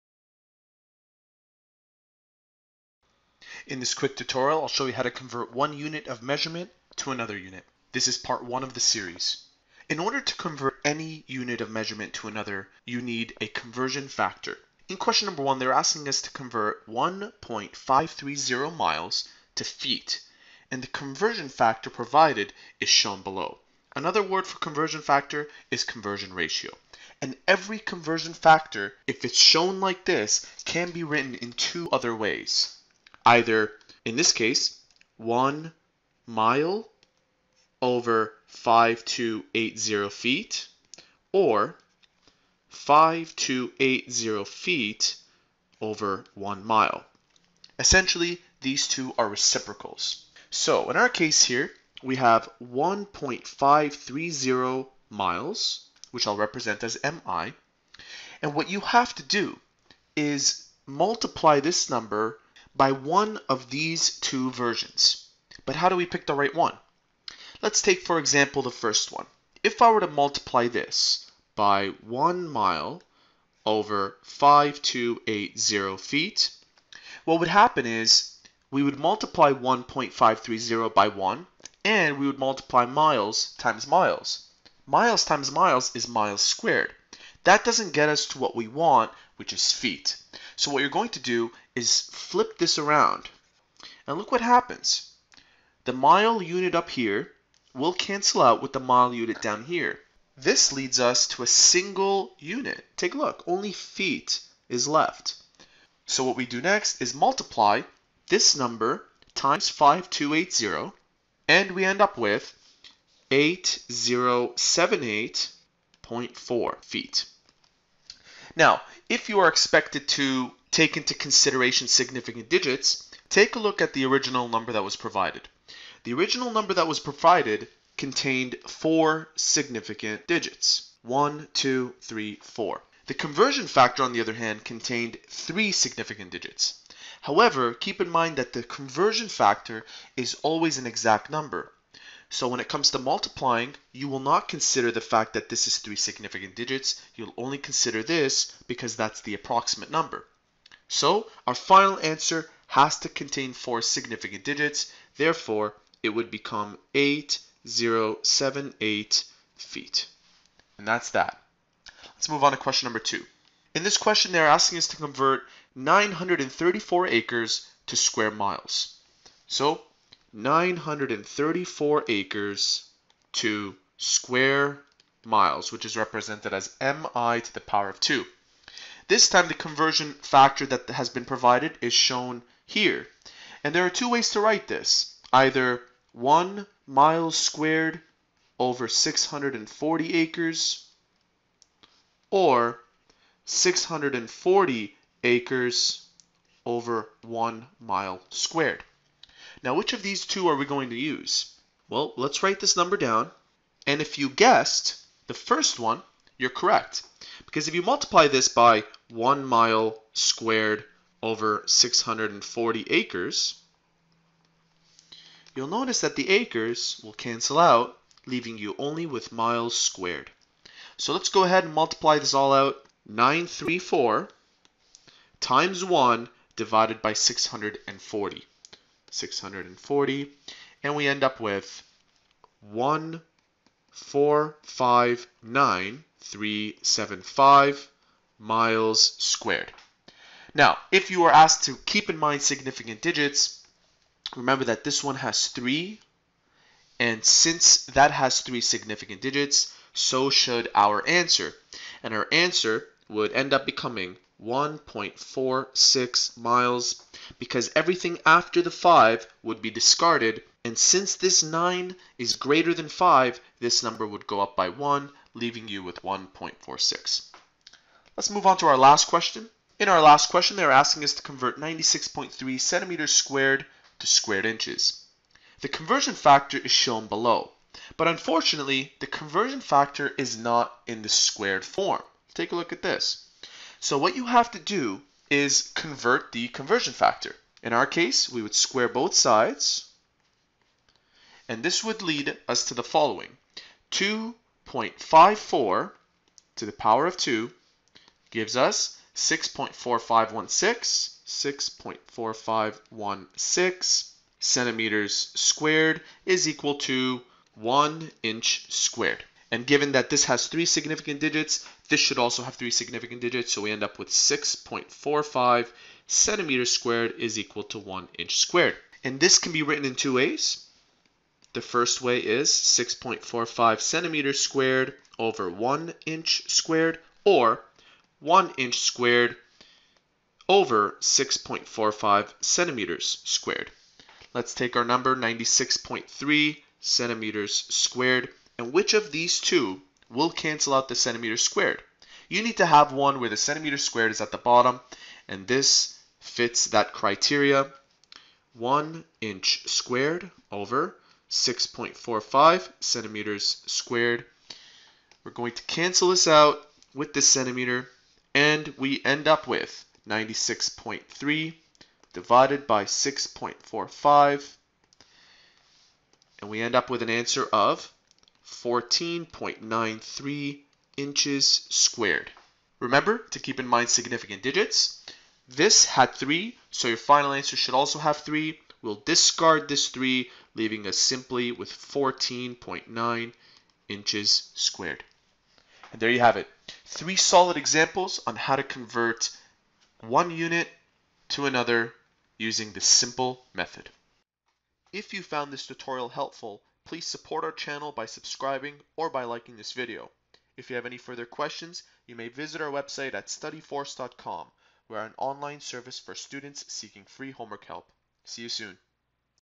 📚 How to convert one unit of measurement to another unit
We convert from one unit of measurement to another by means of a conversion factor.

Questions:

Convert 1.530 miles (mi) to feet (ft).
Conversion factor: 5280 ft = 1 mi

Convert 934 acres to square miles (mi2).
Conversion factor: 1 mi2 = 640 acres

Convert 96.3 cm2 to squared inches.
Conversion factor: 2.54 cm = 1 in